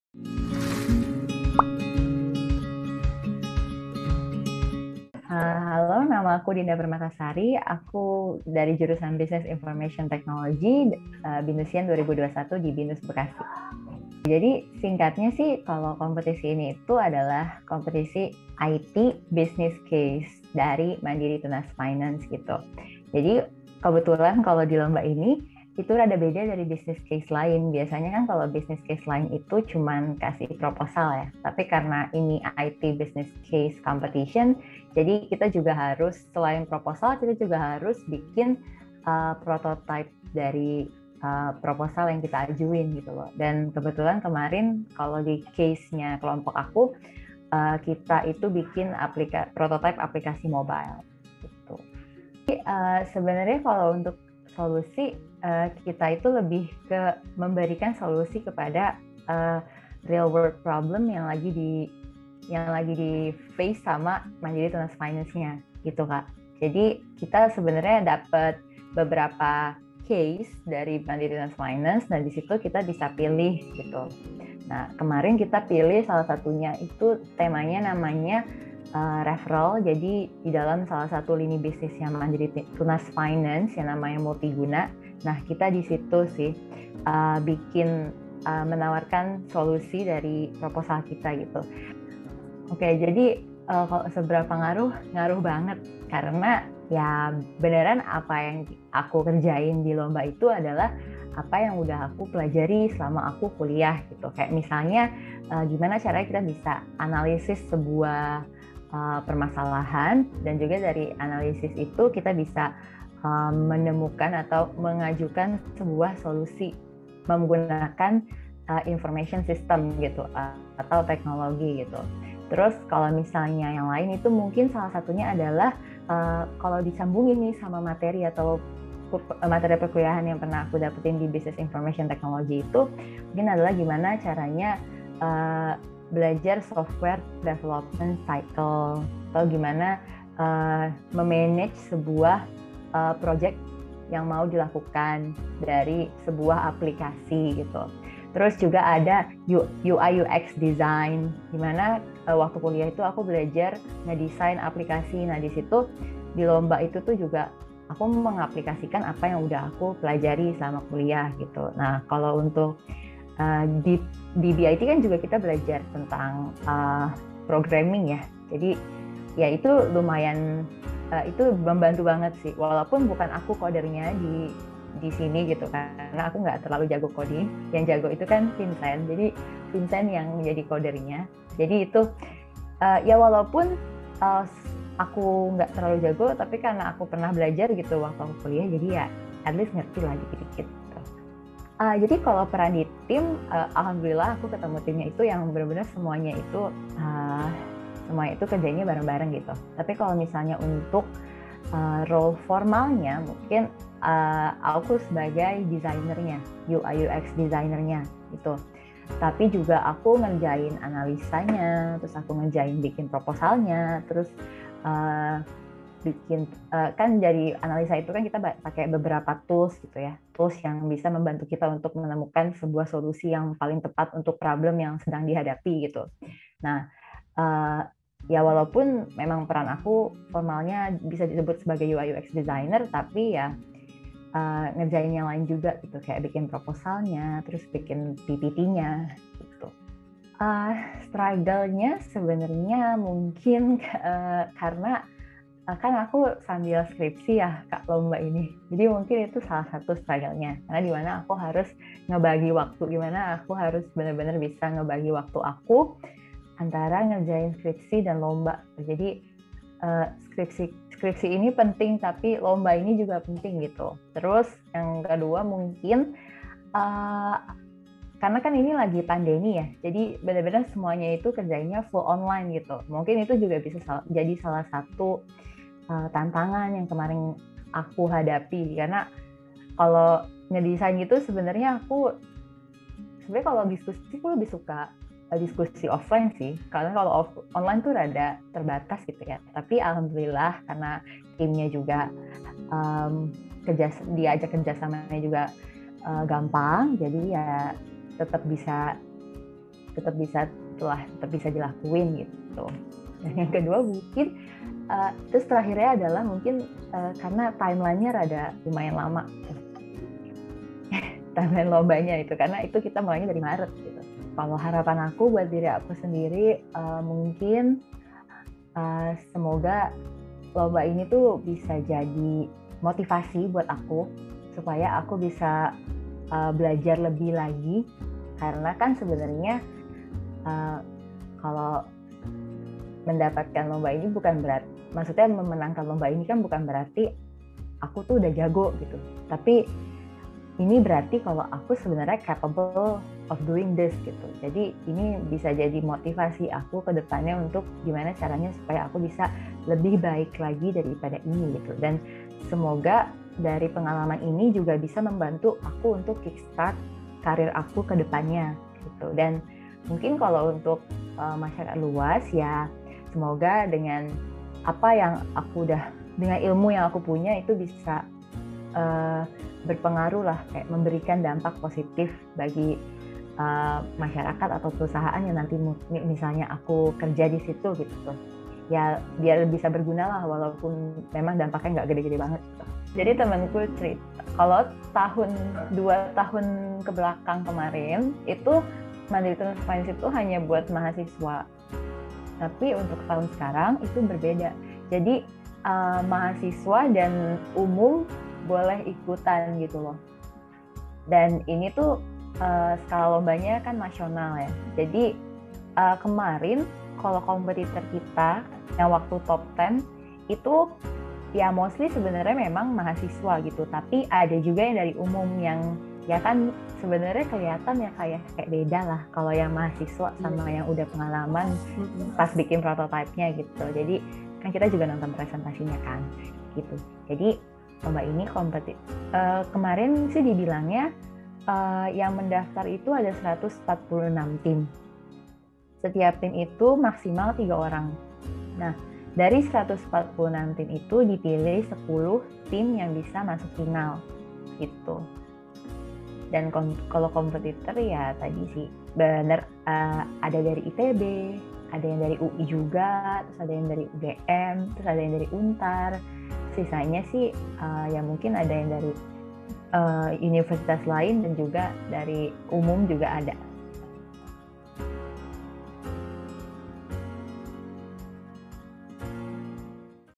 CeritaBINUSIAN 45: Lahirkan Inovasi Multifinance, Dinda P. juarai MTF IT Business Case Competition
Di masa kini, inovasi merupakan denyut nadi yang menopang keberlangsungan dalam industri. Dalam Era Industri 5.0, kemajuan teknologi berlangsung dengan sangat pesat dan bersamaan dengan hal tersebut, kebutuhan-kebutuhan konsumen pun semakin beragam. Masyarakat semakin membutuhkan solusi yang dapat memenuhi kebutuhan mereka secara cepat dan tepat, terutama dalam bidang finance atau keuangan. Dengan adanya kebutuhan-kebutuhan tersebut, generasi Millennial dan Generasi Z, wajib mendapatkan bimbingan dan kesempatan untuk dapat mengasah skill inovasinya.

Menanggapi hal tersebut, BINUS UNIVERSITY, sebagai Perguruan Tinggi Indonesia yang unggul, turut mendukung para BINUSIAN untuk dalam berbagai kompetisi nasional dan internasional yang dapat memperkaya keterampilan inovasi dan pengalaman untuk bekal di industri nantinya. Di tahun 2021 ini, BINUSIAN dari Business Information Technology Program BINUS UNIVERSITY, yakni Dinda Permatasari dan Michael Vincentius Tanakoman, berhasil meraih juara meraih Juara 1 dalam Ajang The 3rd Mandiri Tunas Finance (MTF) IT Business Case Competition. IT Business Case Competition merupakan ajang tahunan yang diadakan oleh PT Mandiri Tunas Finance yang bertujuan untuk melahirkan inovasi-inovasi di bidang multifinance.

Acara ini selain bertujuan untuk menyelesaikan permasalahan di bidang multifinance melalui teknologi digital, juga bertujuan untuk mendorong para inovator di Indonesia untuk bersama – sama memajukan kemajuan industry multifinance dan tentunya untuk mengembangkan kemampuan para inovator itu sendiri untuk menjadi lebih baik. Di tahun ini, kompetisi tidak hanya diikuti oleh mahasiswa, namun juga dapat diikuti oleh para professional muda.  Tim dari Business Information Technlogy Program BINUS UNIVERSITY sendiri, yakni Tim Dewa Api, berhasil membawa pulang total hadiah sebesar Rp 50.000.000 setelah berhasil memberikan solusi Mobile Application dalam kompetisi ini.

Menjadi pemenang dalam kompetisi tingkat Nasional ini, tentunya bukan hal yang mudah. Salah satu anggota Tim Dewa Api, Dinda Permatasari, harus menyeimbangkan kewajibannya menyelesaikan skripsi sekaligus mengikuti kompetisi. Begitu pula dengan Vincent, dirinya banyak menemukan tantangan dalam proses coding atau pemrograman dalam membuat prototipe dari solusi yang ditawarkan. Meski begitu, baik bagi Dinda maupun bagi Vincent, bekal yang didapatkan selama berkuliah di Business Information Technology Program, sangat mendukung mereka untuk mengatasi seluruh tantangan yang dihadapi dan bahkan berhasil menjadi Juara 1.

Selamat kepada Dinda dan Vincent dari Business Information Technology Program BINUS UNIVERSITY! Diharapkan prestasi ini dapat menginspirasi inovasi-inovasi bagi para BINUSIAN dan generasi muda lainnya di Indonesia.

Follow our Instagram @binusofficial